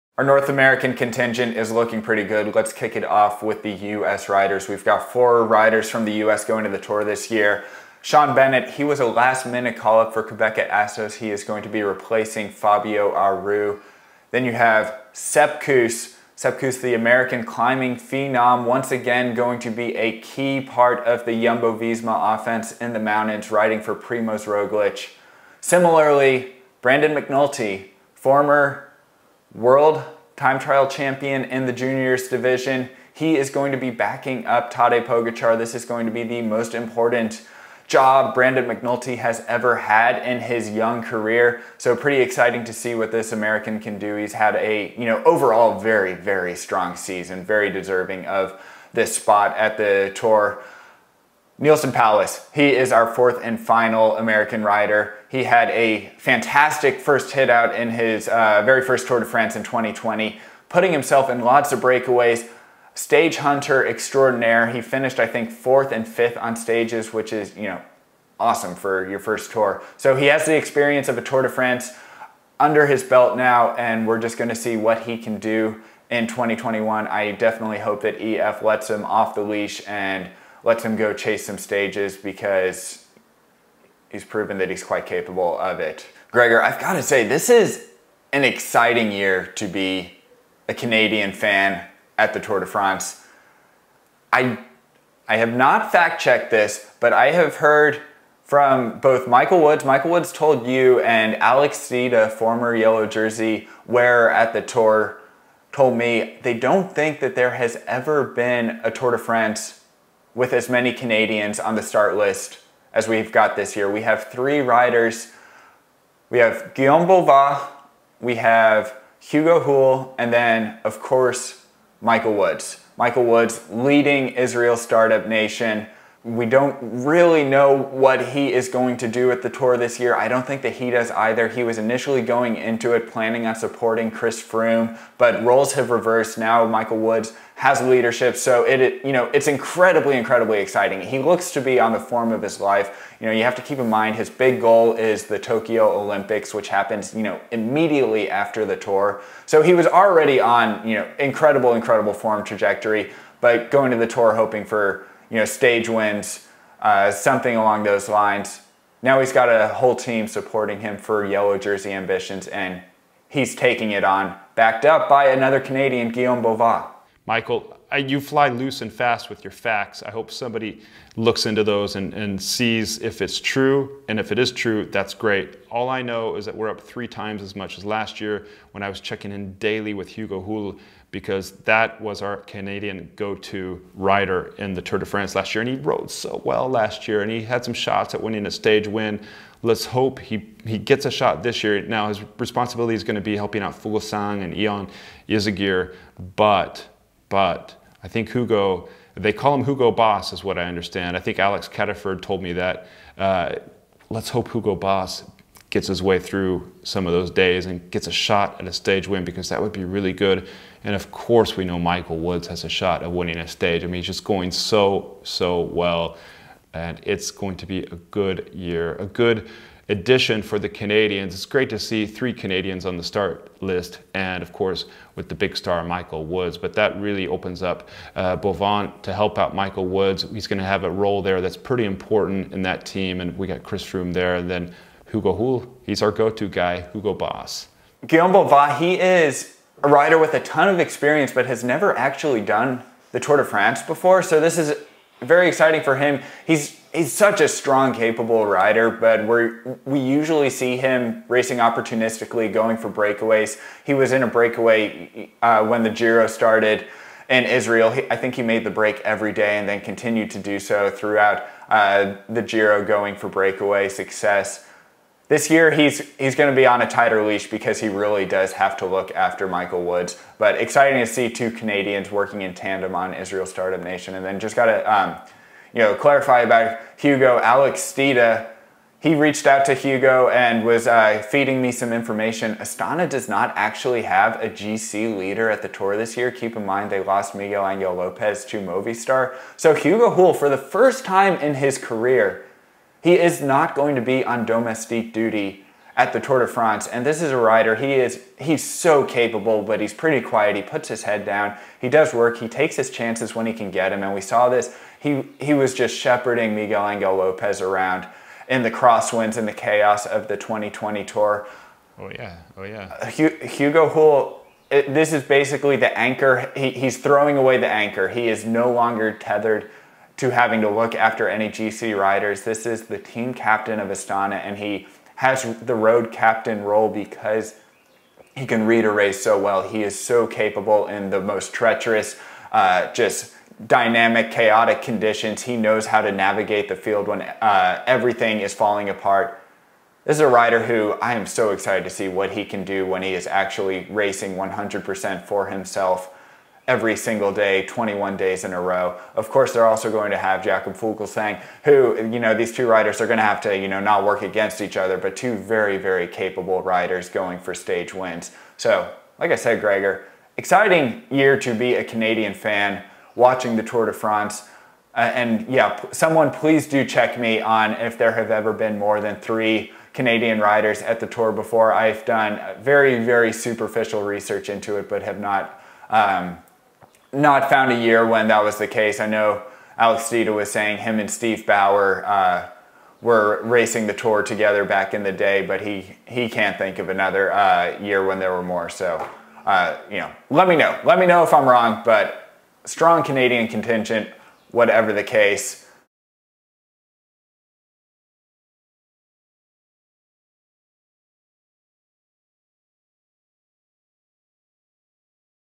North American Tour de France Contingent: A Force To Be Reckoned With
presented by @Zwift
Michael and Gregor talk about the US and Canadian riders lining up in Brest for the start of the 2021 Tour de France, including Mike Woods, Sepp Kuss, Sean Bennett, Neilson Powless, Hugo Houle, Brandon McNulty, and Guillaume Boivin.